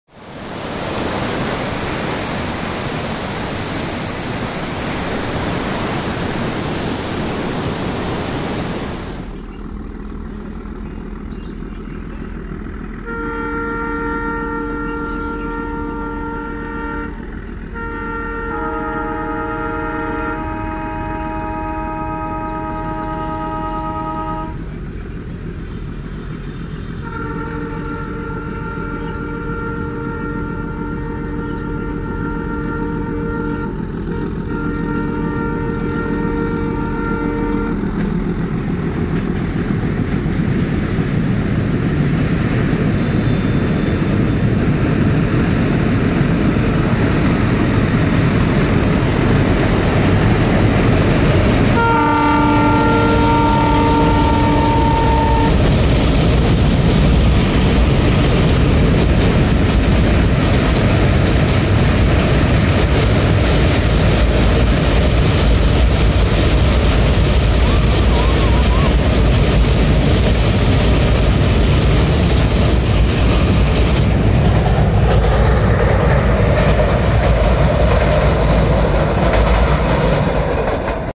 Mailani - Bahraich MG Train across the Kailshpuri Dam Canal Bridge
One of the last few functional meter gauge railway route in Indian Railway, the Bahraich - Mailani and Bahraich - Nepalganj Road in Uttar Pradesh is a very charming railway route that runs through 2 wild life sanctuaries enroute. Here in this video is the Mailani - Bahraich Passenger across the Canal bridge near Kailashpuri Dam, Uttar Pradesh. More videos from Dudhwa National Park coming up shortly.